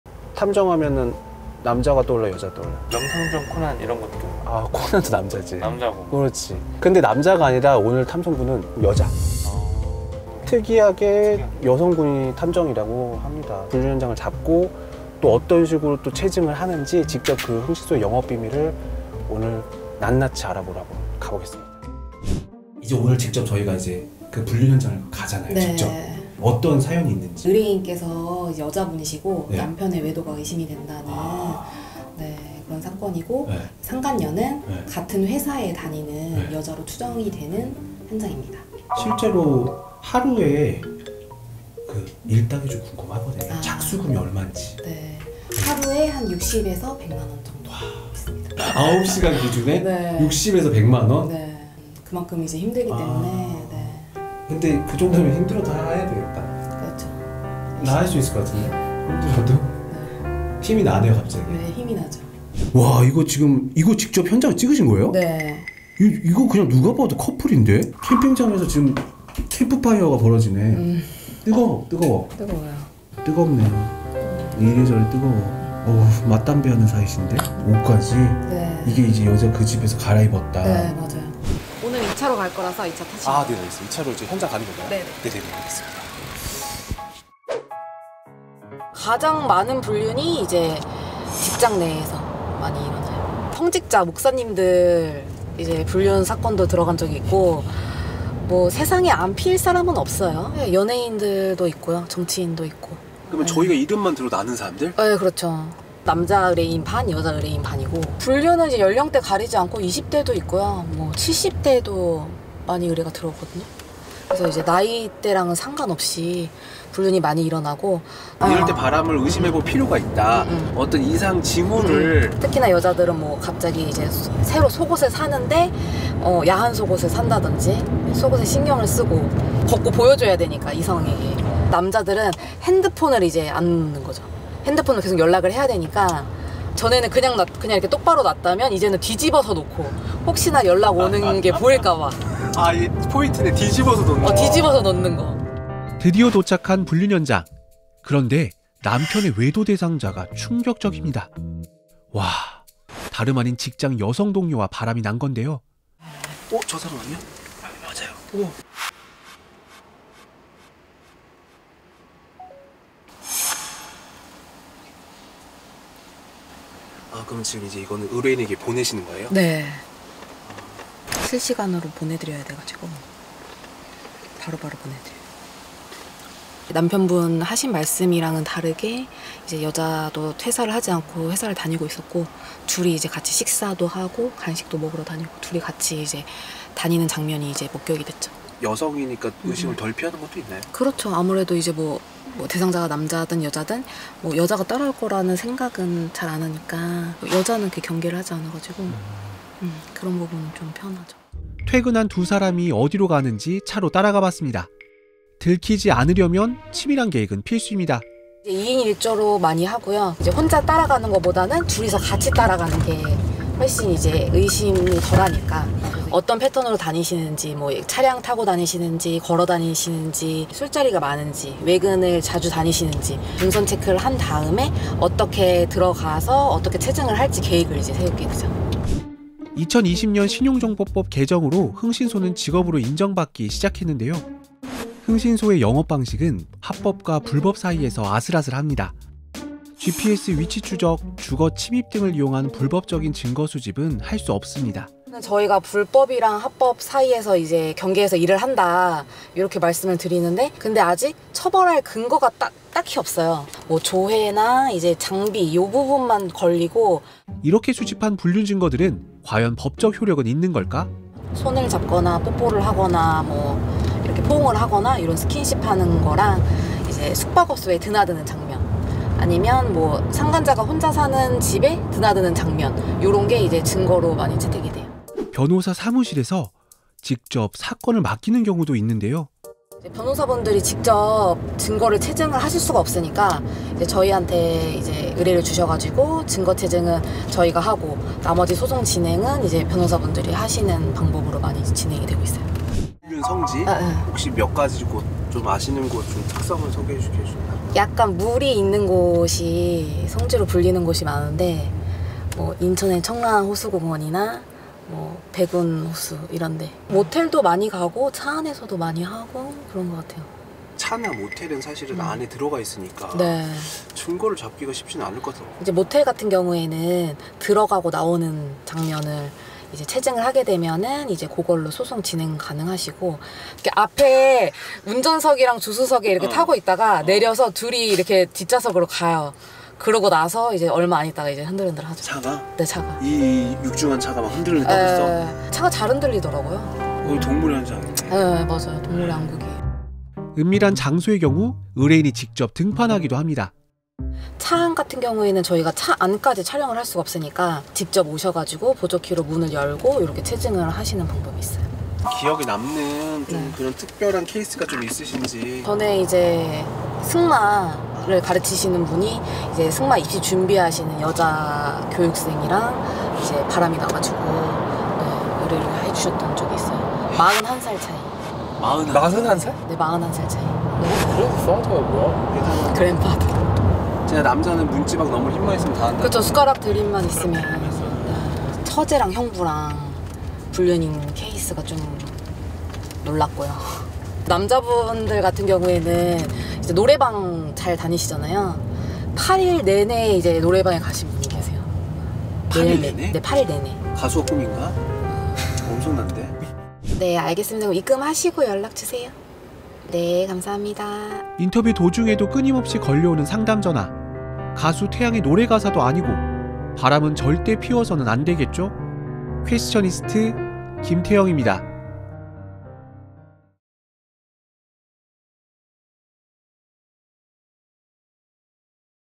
리얼 실제상황ㄷㄷ 경악스러운 요즘 불륜 특징! 탐정과 함께하는 흥신소의 하루
🔎 배우자의 불륜 증거 수집해드립니다 💔
이런 흥신소 광고 본 적 있으신가요?
흥신소와 관련된 불륜증거 수집방법, 의뢰비용까지 모두 파헤친다!! 💸

🕵️‍♂️ 더레드의 주서빈 탐정님을 인터뷰했습니다